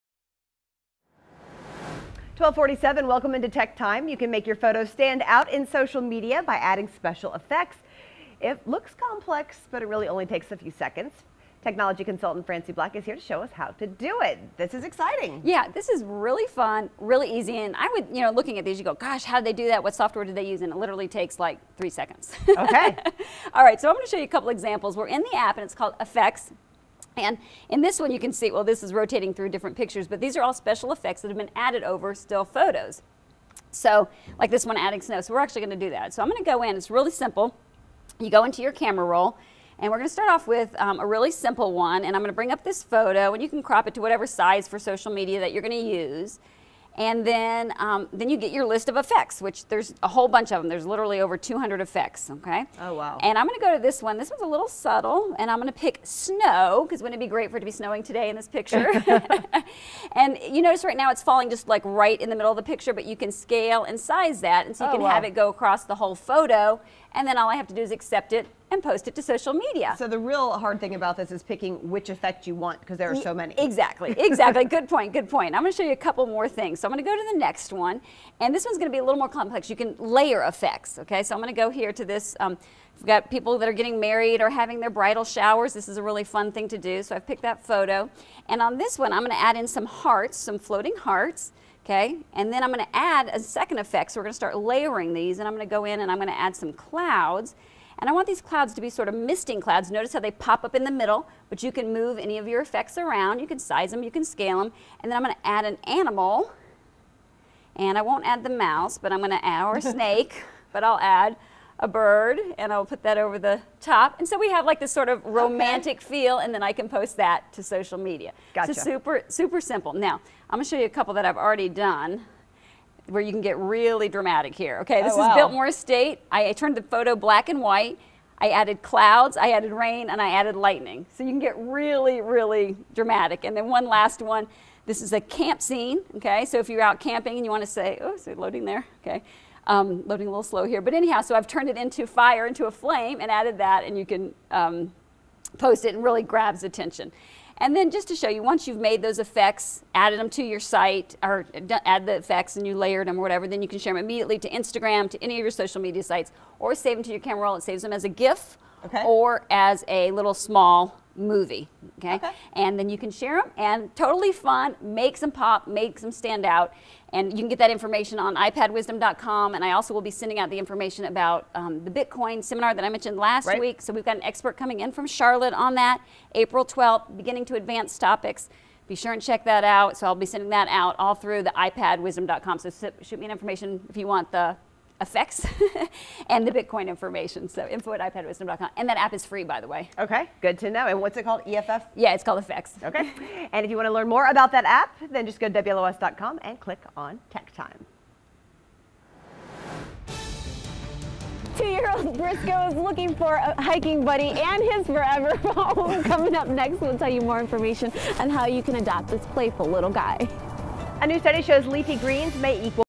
How to add animated special effects to photos in seconds using XEFX app
If you want to make your photos stand out in social media posts, the new XEFX app allows you to add animated special effects to your pictures in just seconds. You can easily add falling snow, active rain drops, blowing clouds, lightening strikes, floating hearts, moving animals and so much more.  It is easy!